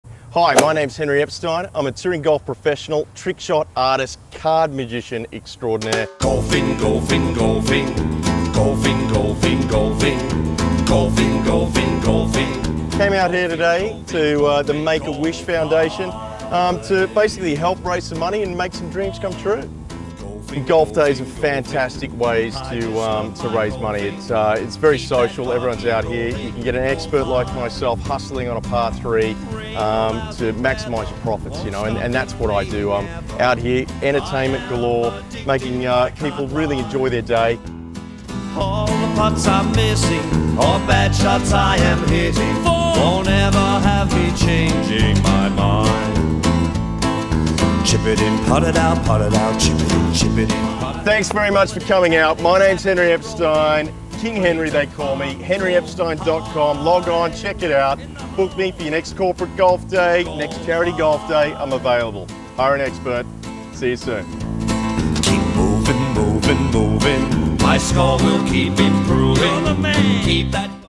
Golf Day Entertainment
Introducing Henry Epstein AKA King Henry Corporate Golf Day Entertainment. This is a little example of what I do best, the joy I can bring, and how I can make your next golf day stand out from the rest.